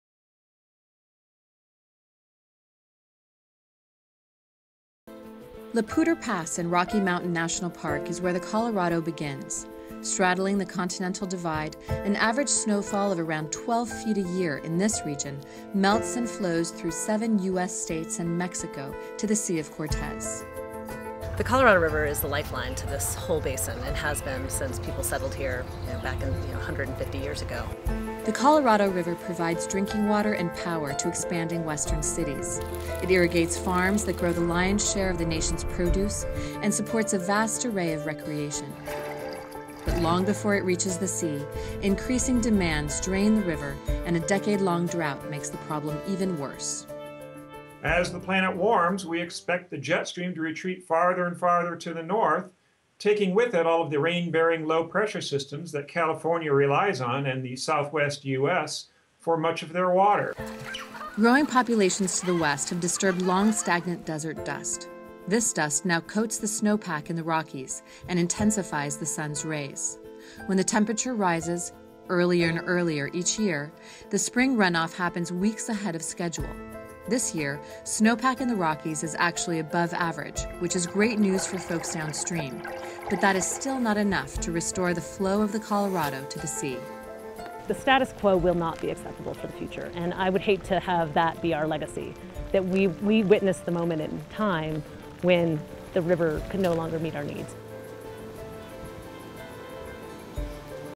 Colorado River Series: Headwaters
Part One of a series on the many challenges Climate Change is creating for those who rely on the Colorado River Basin.